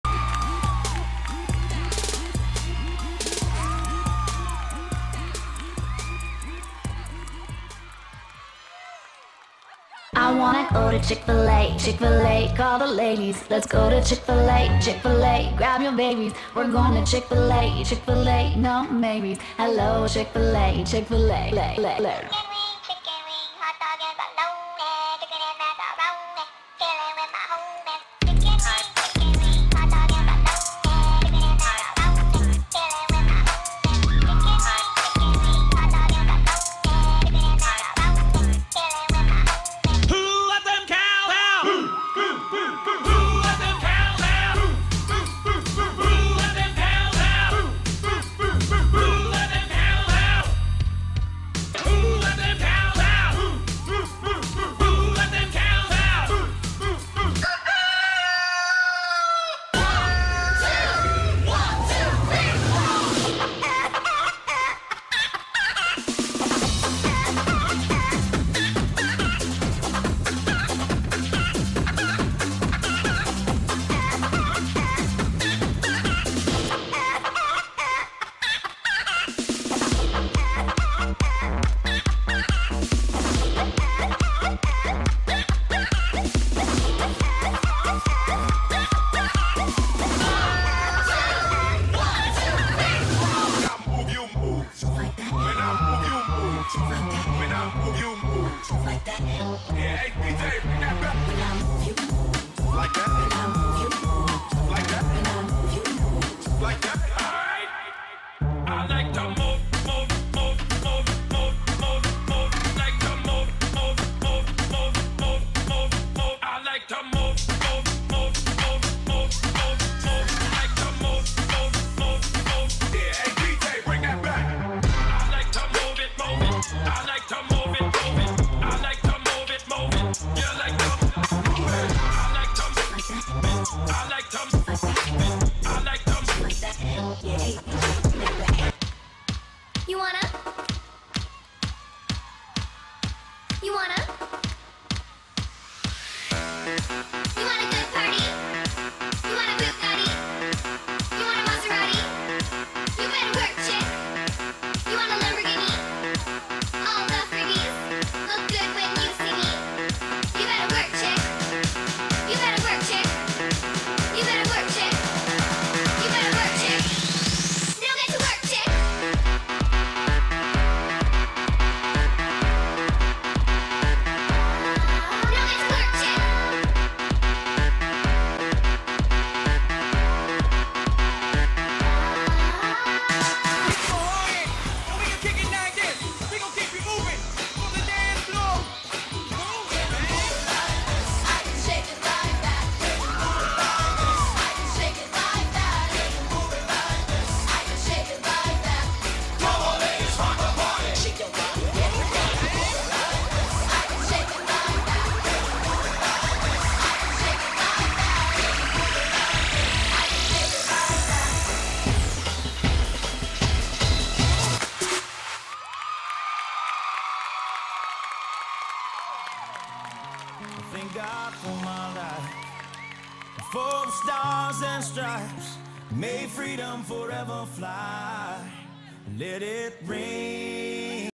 Midwest Elite Dance Center- Chick Fil A
Age 6 Mini Novice Open Line
1st overall
Revolution Pensacola, FL Nationals 2023